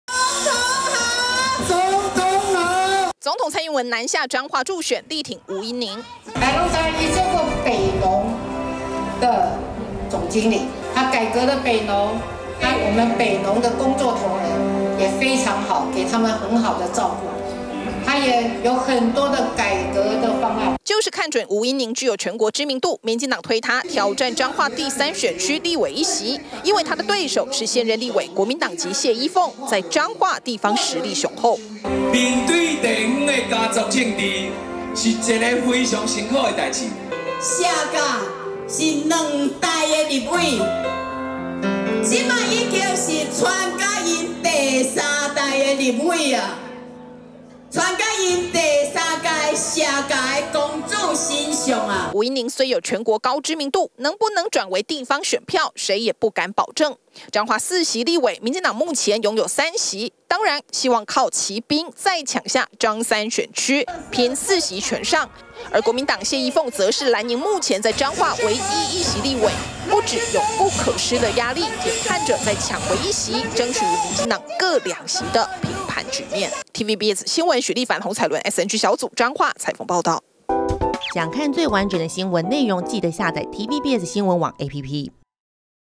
總統力挺吳音寧! 綠盼彰化立委4席全上｜TVBS新聞
彰化第三選區立委選舉，民進黨立委參選人吳音寧要挑戰的是「在地政治世家」國民黨籍現任立委謝衣鳳，總統蔡英文特地南下站台助選！